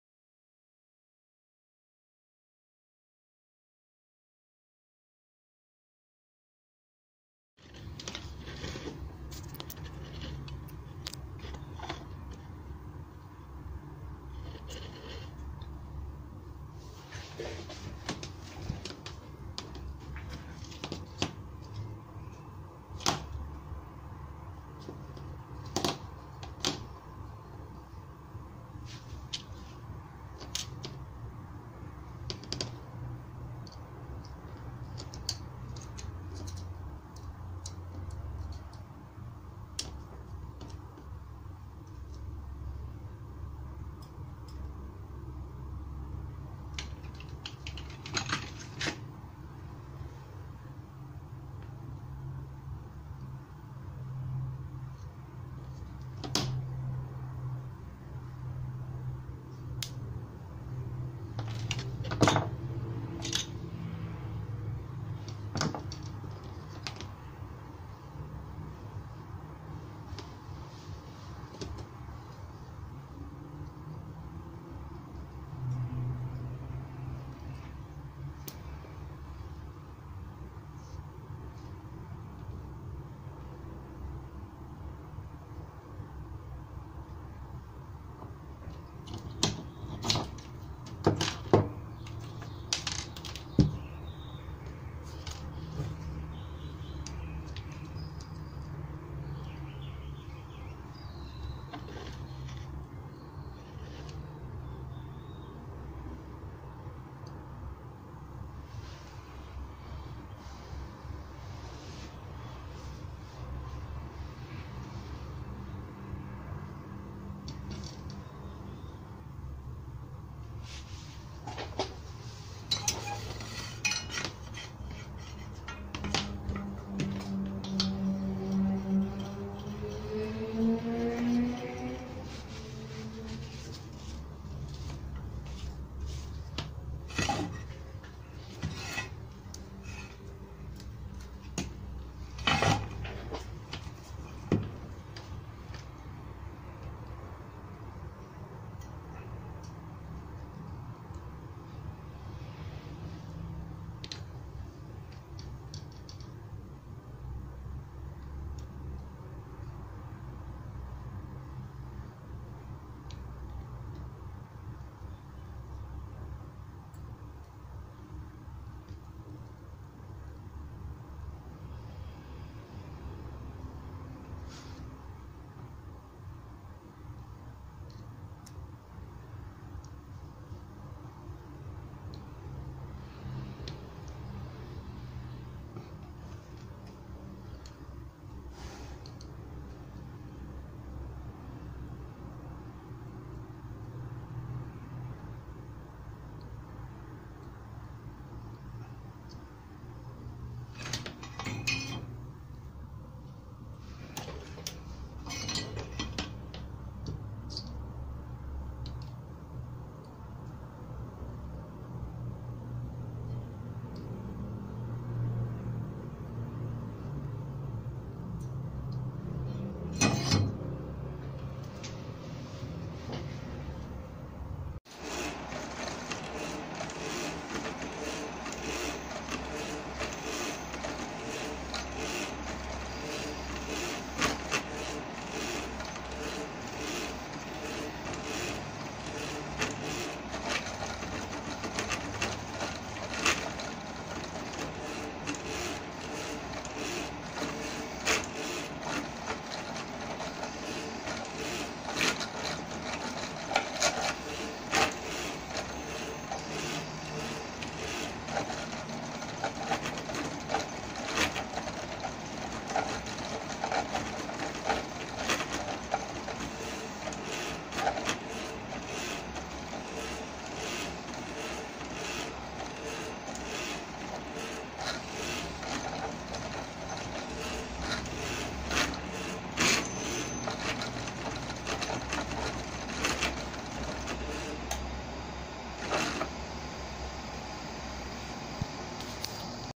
Arduino como soldar display 2x16 lcd
Si te ayudó a reparar , aprender, o te inspiró a construir algo, podes ayudar con una pequeña colaboración.
Instagram Como soldar una tira de pines a un display de 16x2